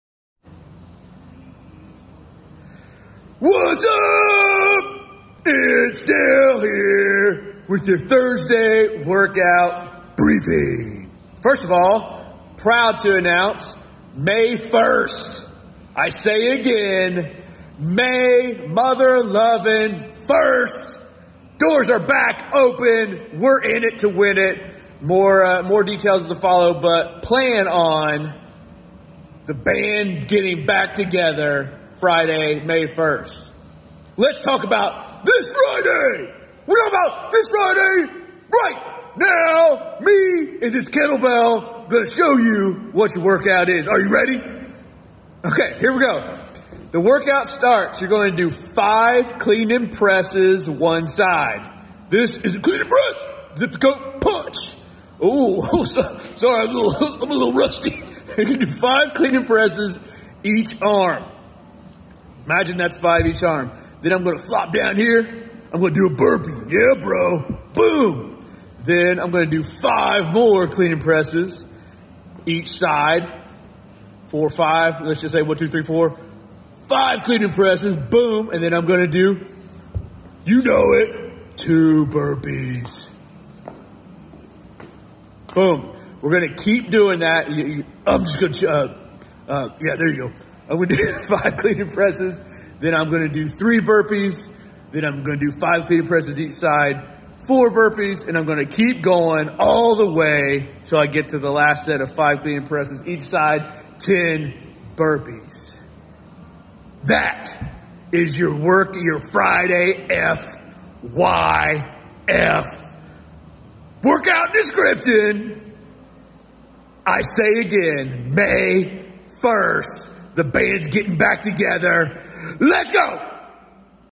At Home Kettlebell Workout - workout 27
workout is:
For time:
5/5 clean and press
1 x burpee
5/5 clean and press
2 x burpees
5/5 clean and press
10 x burpees